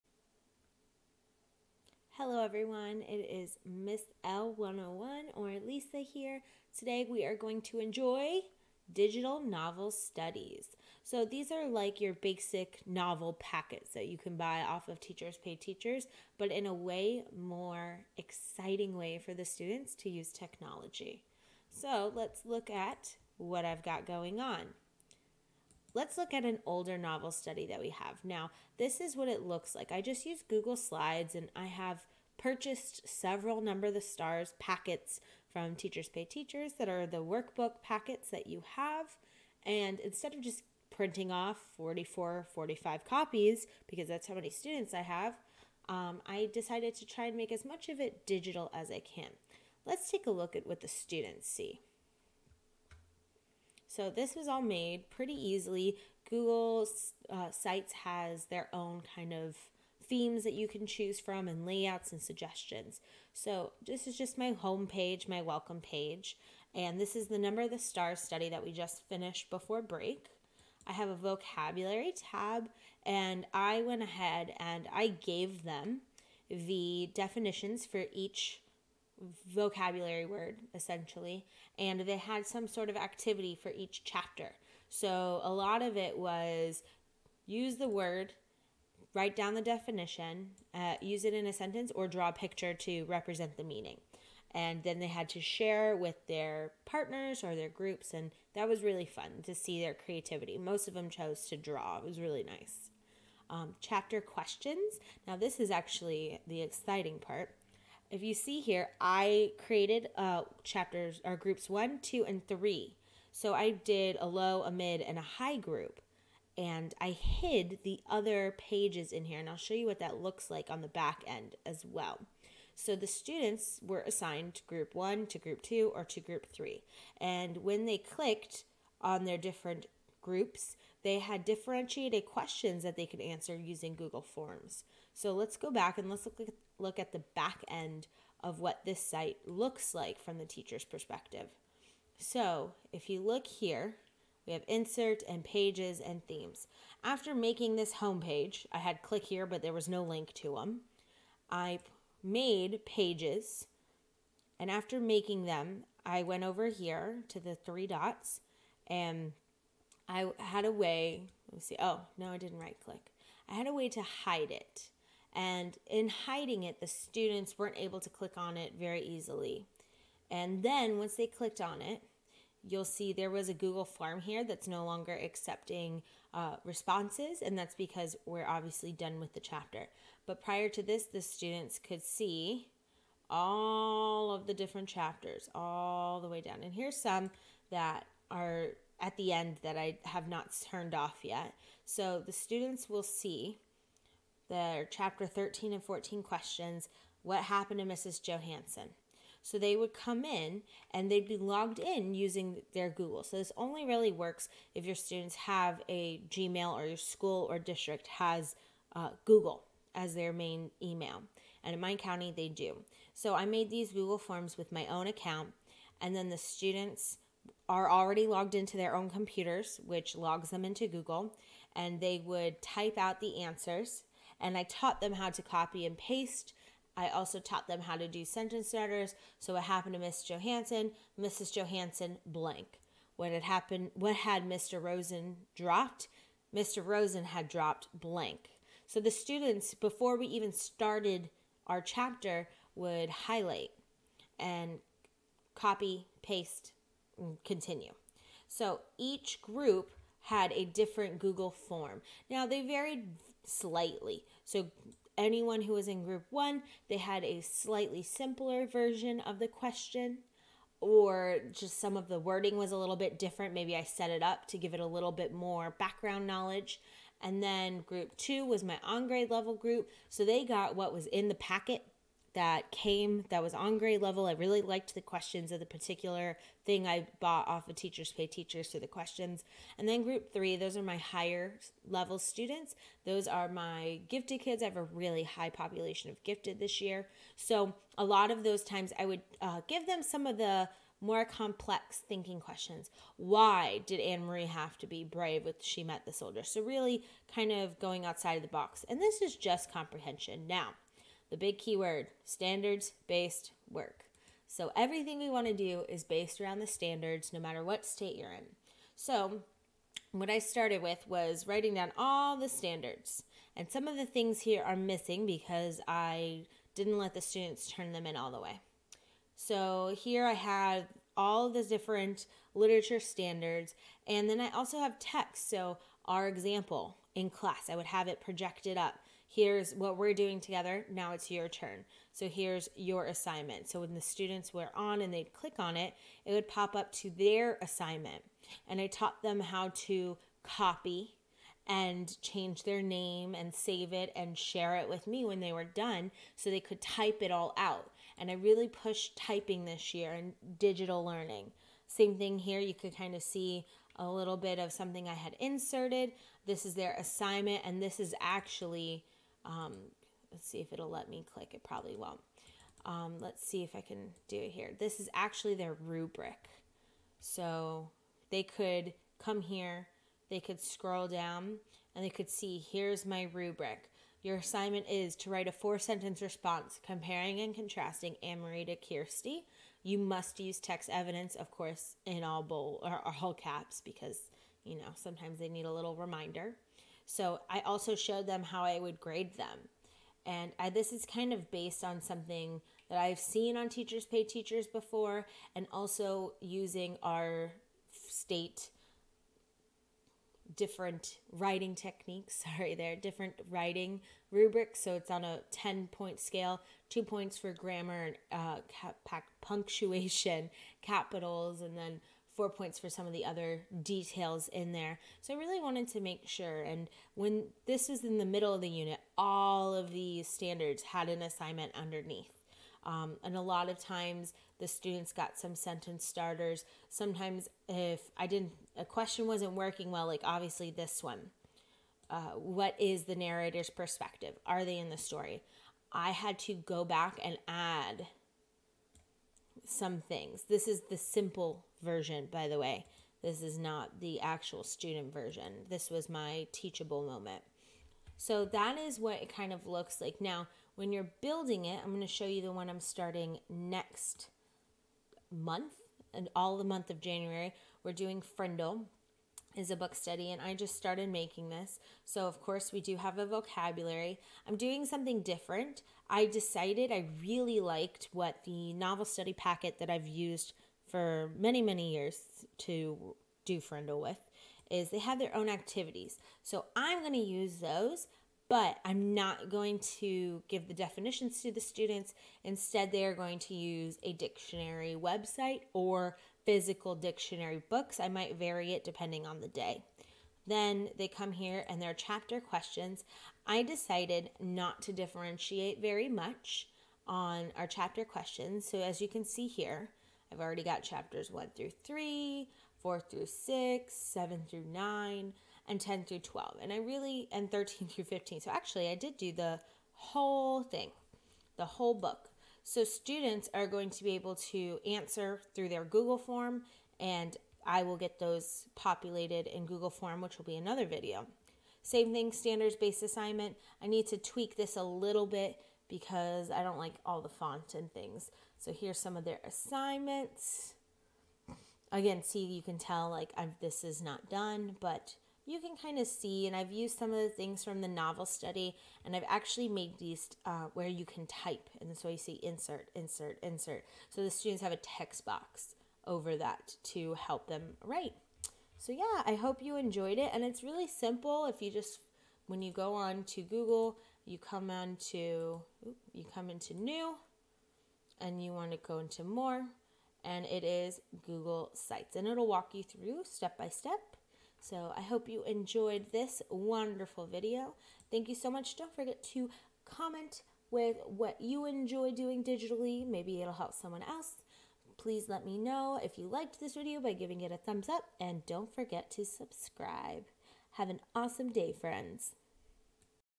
Digital Novel Studies
How to engage your students with a digital novel study!

Products used:
Google Sites